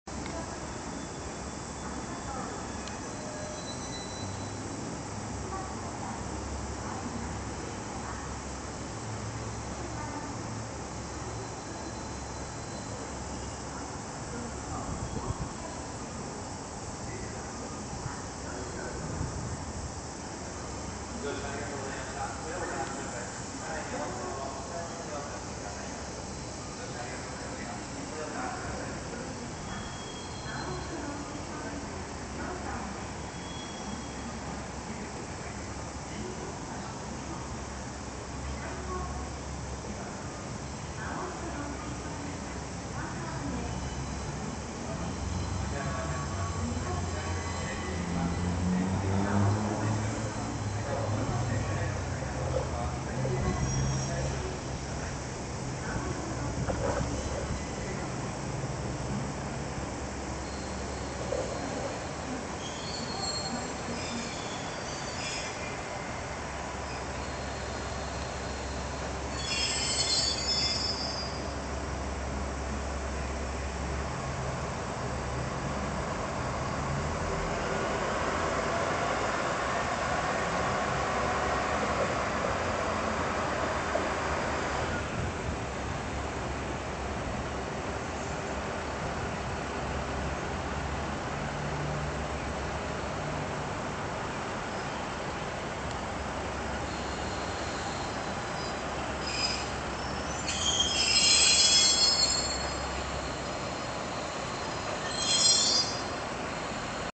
JR豊岡駅に入線するキハ47
兵庫県豊岡市のJR豊岡駅にて撮影。